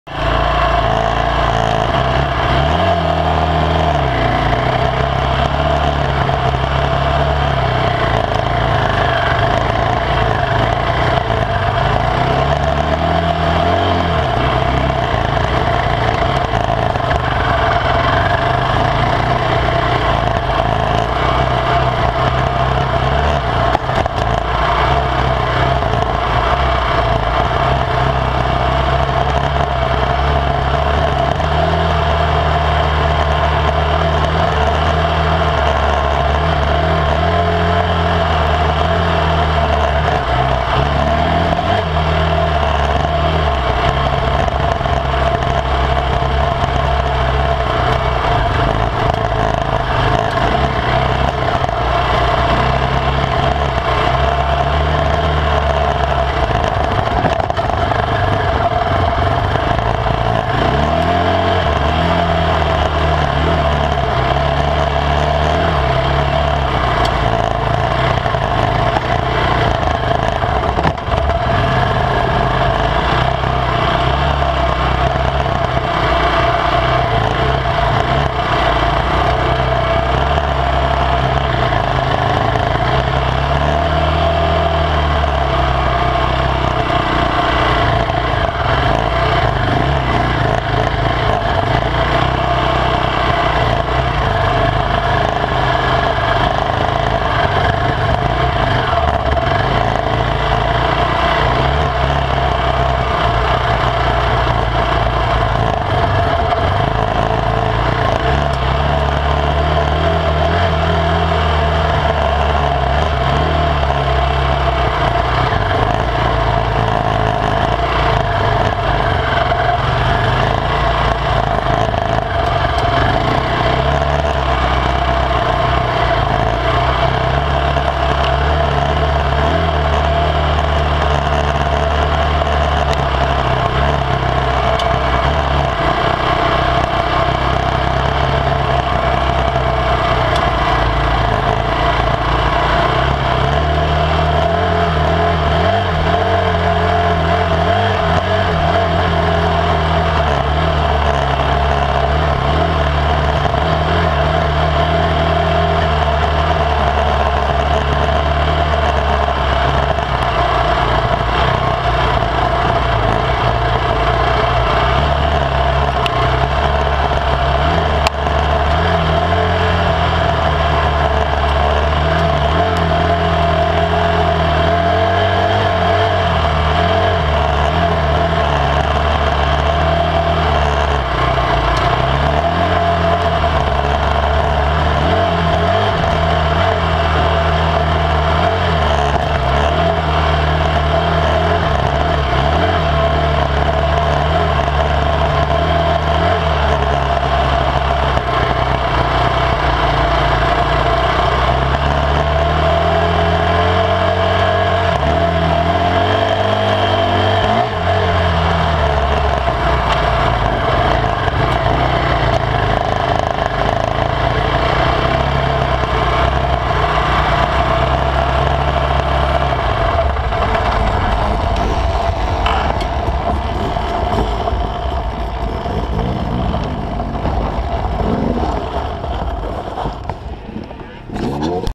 RRMMC Spring Poker run flagging part 2
Trick is to hang the flag (arrow) with a bow in it so it's easy to pull down on Sunday when we sweep the course